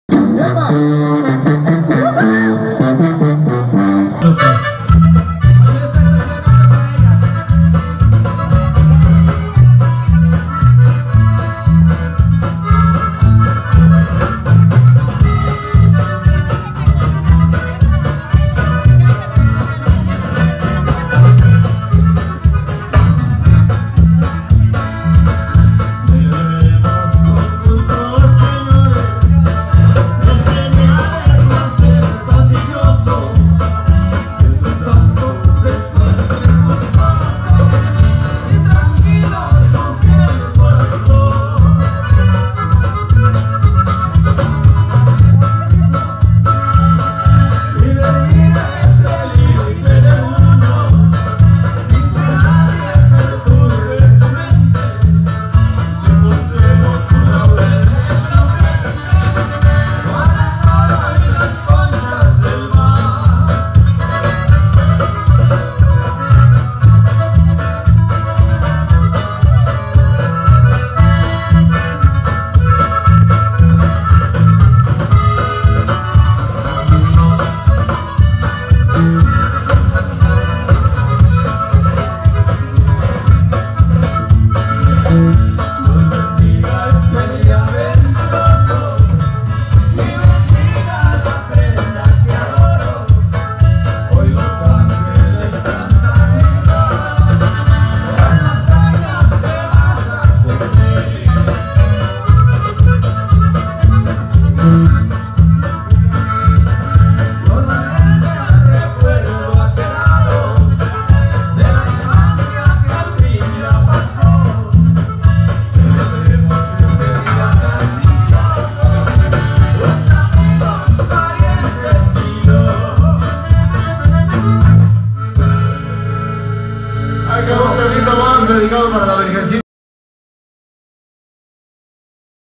La Proyeccion De Rio Verde En Vivo
hay nomas que chulada de musica mas ambiente mas exitos desde San Luis Potosí con el grupazo la proyección de rio verde en vivo